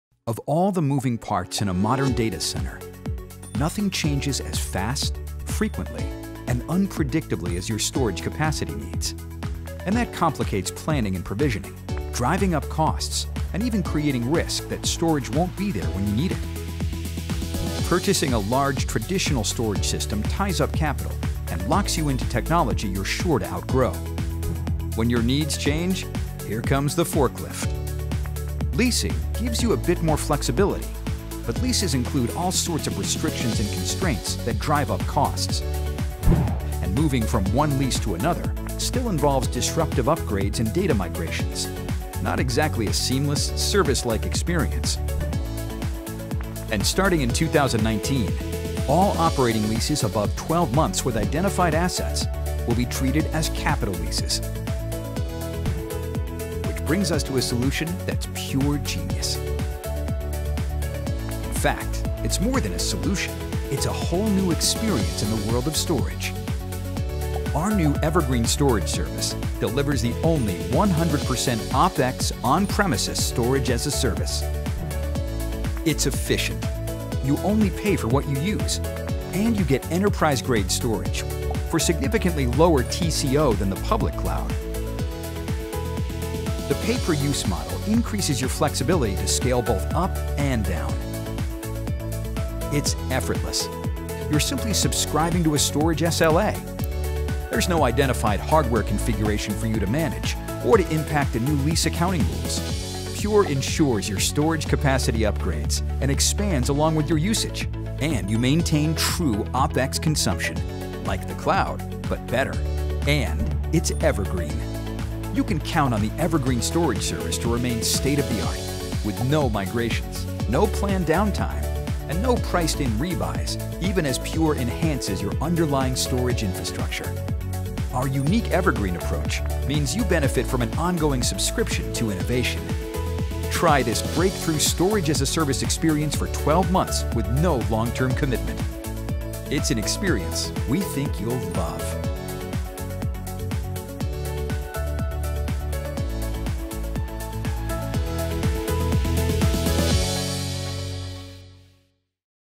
Storage-as-a-Service (STaaS) for Private and Hybrid Clouds - ES2
See how Evergreen Storage Service (ES2) can address your unpredictable storage needs with enterprise-grade, elastic, scalable Storage-as-a-Service (STaaS) that aligns with your business needs.